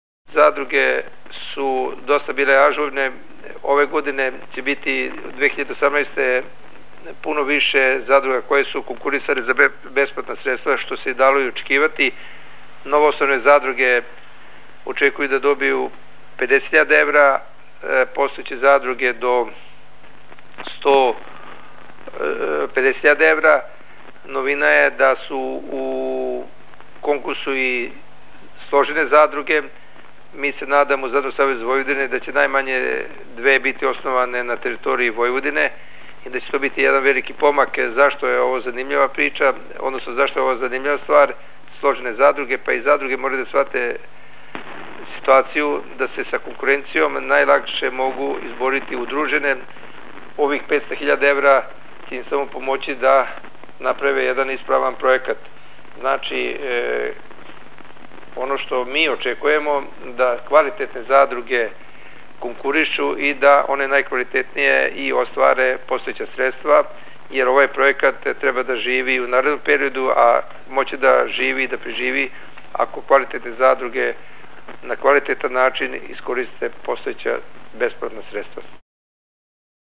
Fono Radislav Jovanov
Kabinet ministra bez portfelja zadruženog za regionalni razvoj i koordinaciju rada javnih preduzeća raspisao je javni konkurs za realizaicju Programa podrške sprovođenju mera ravnomernog regionalnog razvoj kroz učešće države u ekonomsko-finansijkim podsticajima za zemljoradničke i poljoprivredne zadruge u 2018. godini. Konkurs je otvoren do 15. avgusta a za složene zadruge do 15. septembra 2018. godine, kaže predsednik Zadružnog Saveza Vojvodine Radisav Jovanov